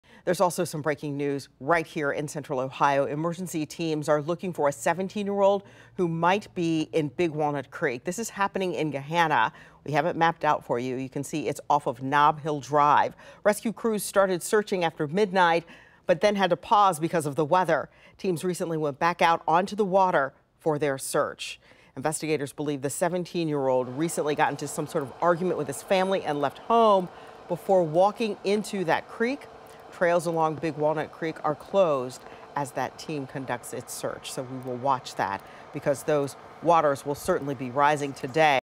Crews searching for 17-year-old who reportedly walked into Big Walnut Creek
According to a spokesperson for Mifflin Township, crews are looking in the area of Academy Park in Gahanna.